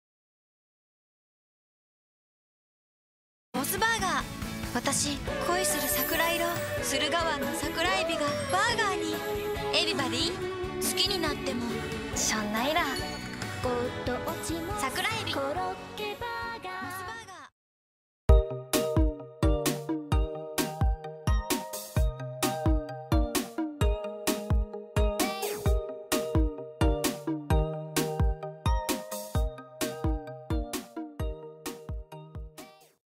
Commercial of a very pretty " Koizumi Moeka" appearance of a dear hairstyle
A very cute girl of a very dear hairstyle "Koizumi Moeka" is appearing. A commercial of a this place burger of "Mos Food Services" "small shrimp croquette burger". It's also the good picture I feel like eating shortly.